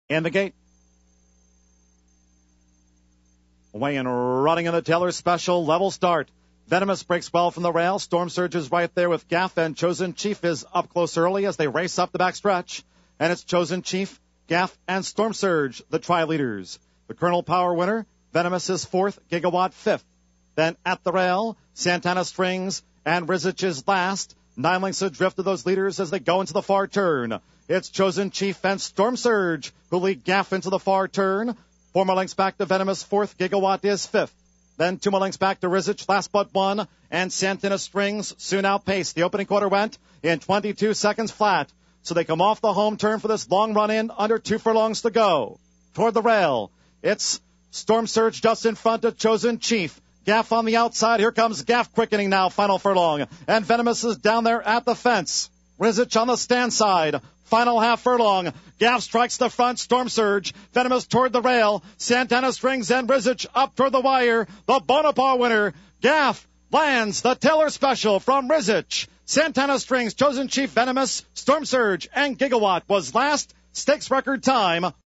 FAIR GROUNDS, 2007-02-10, Race 5
Entries: Thoroughbred Race, FAIR GROUNDS, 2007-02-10, Race 5:
      (Pgm-Runner-Jockey-Finish)
       1-Venomous (TX), Perrodin, E, finished 5;
       2-Santana Strings (FL), Talamo, J, finished 3;
       3-Gaff (KY), Meche, D, finished 1;
       5-Storm Surge (KY), Velazquez, J, finished 6;
       6-Chosen Chief (KY), Albarado, R, finished 4;
       7-Gigawatt (KY), Graham, J, finished 7;
       9-Wrzeszcz (KY), Melancon, L, finished 2;